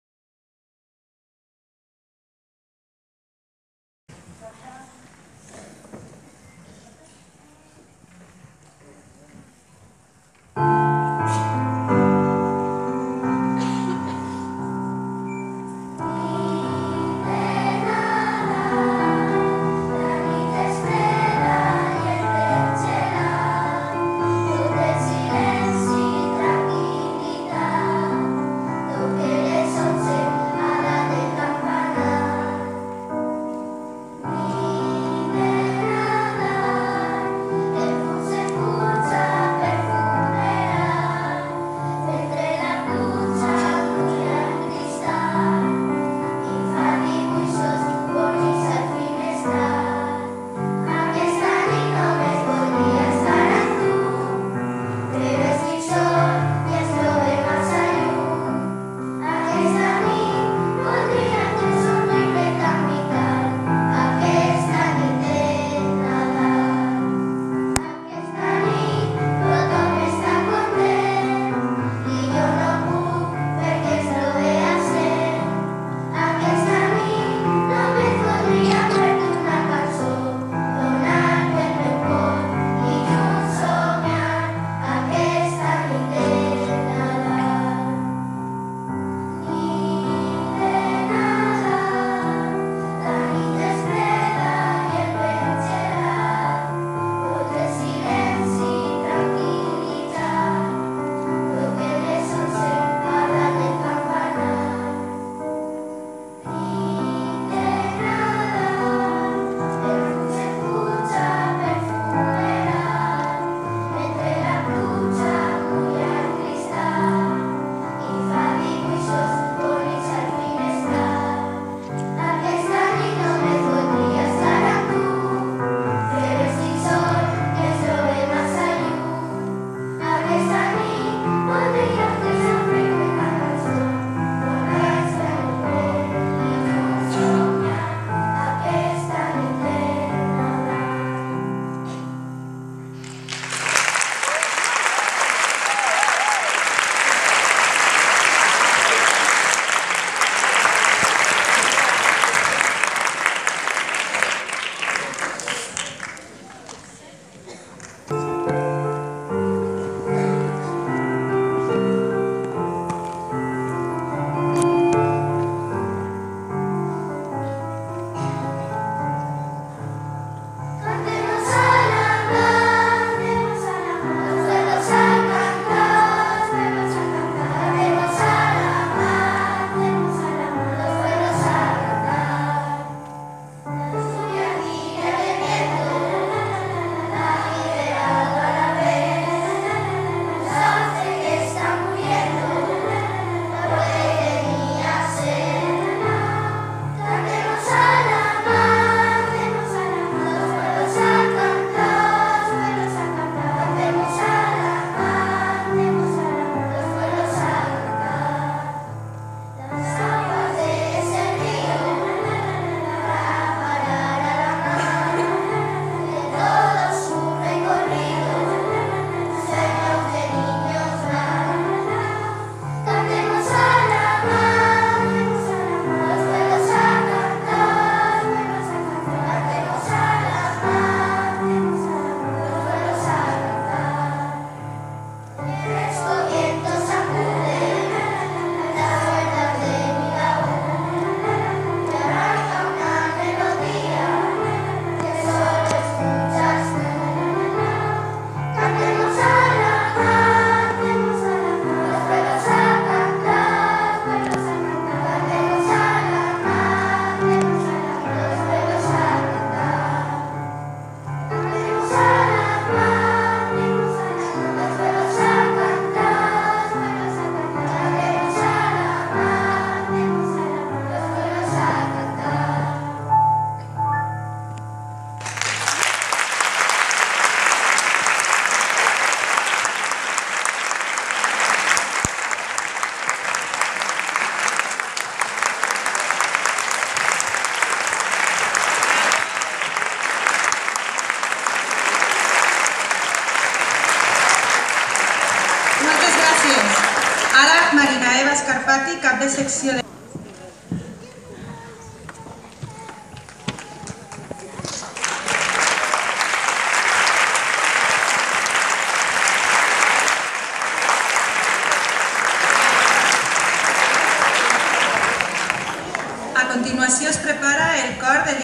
CEIP SERRERÍA. III Trobada de cors escolars
Trobada de Cors escolars per Nadal
Espai La Rambleta.
Ajuntament de València.
28 de novembre de 2018